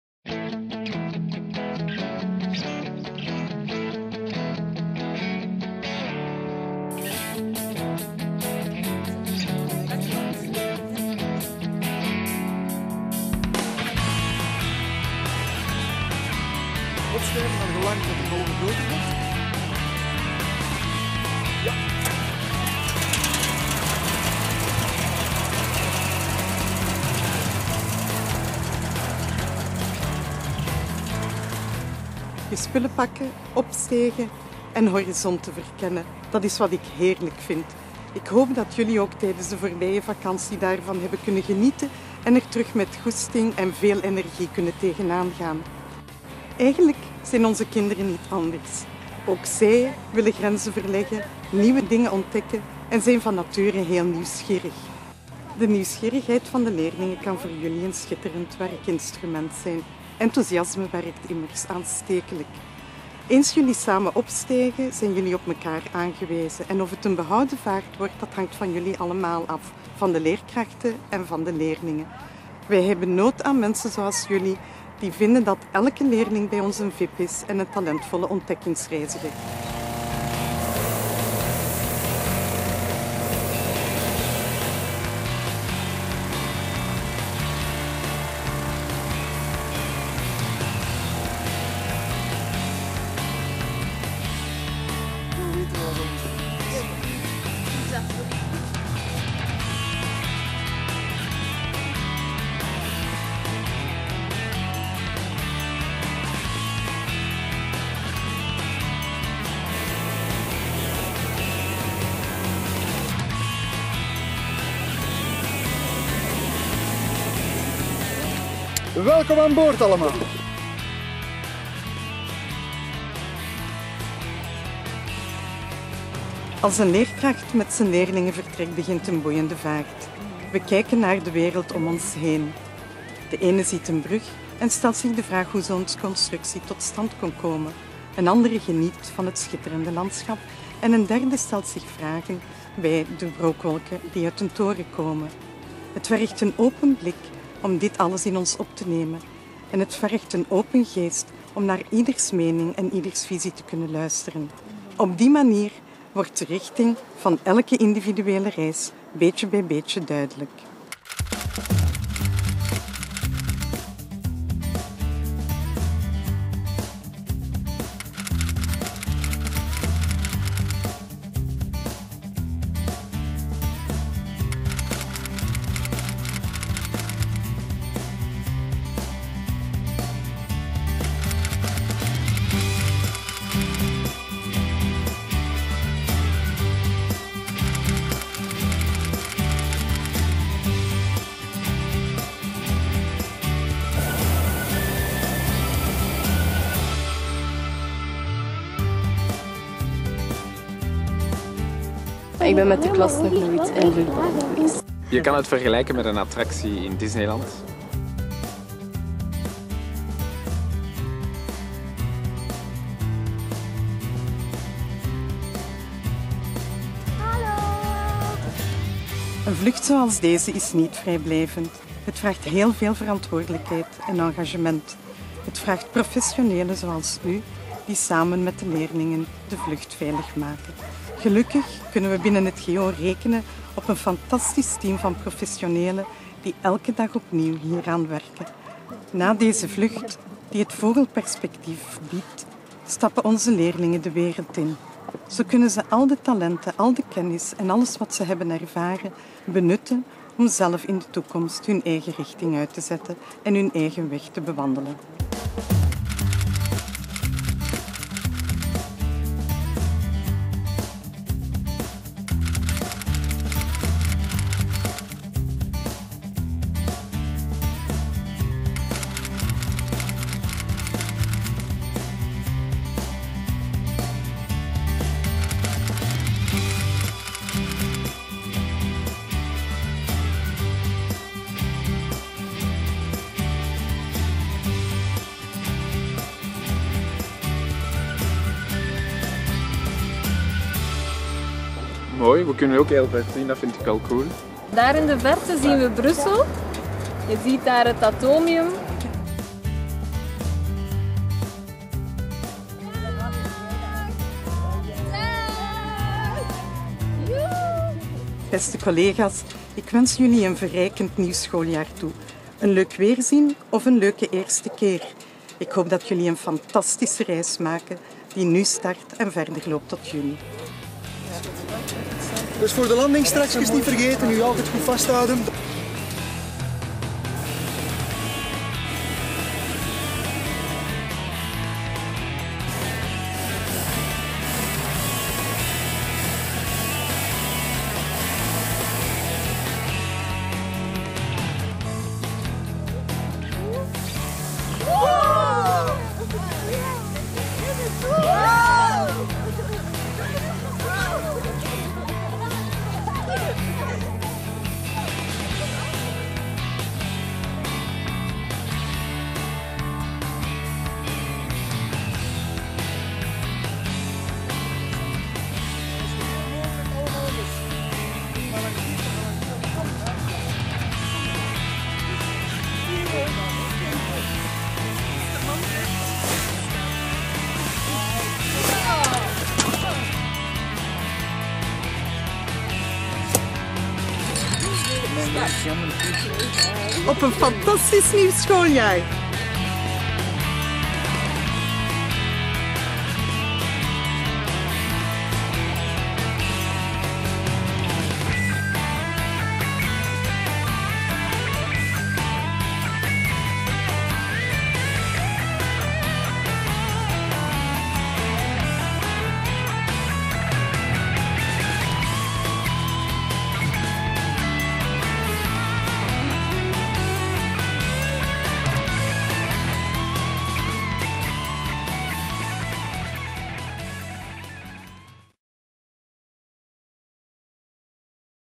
1 septemberboodschap Raymonda Verdyck, afgevaardigd bestuurder GO!, schooljaar 2010-2011
Samen met Milan, Ezra, Lisa, Odile, Willem, Laurens, Mias en Cynthia maakte Raymonda Verdyck een prachtige vaart in een luchtballon. Zij brengt u vanuit de lucht haar septemberboodschap met zicht boven het Hageland.

Elke passagier heeft een droom en maakt zijn eigen vlucht.
Raymonda Verdyck wil samen met alle collega's in het GO! bouwen aan open scholen met een open geest om de dromen van al onze kinderen waar te maken.